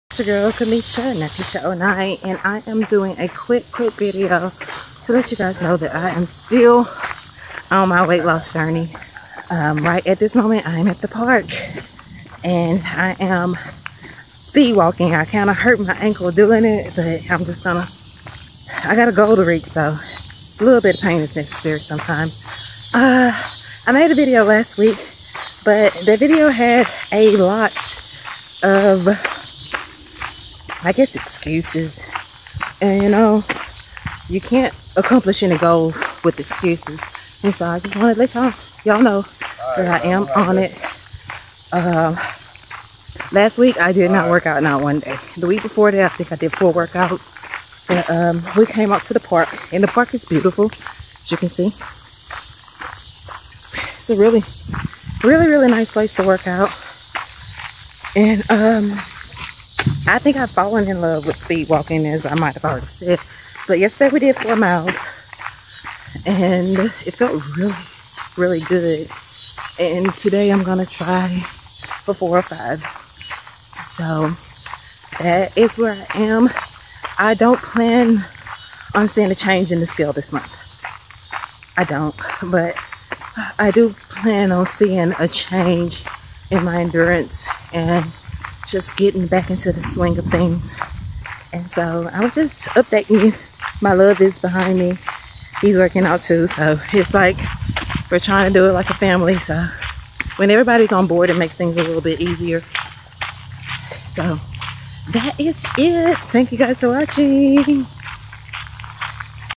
Speed walking
Just updating you guys pin my progress and my newfound live for speed walking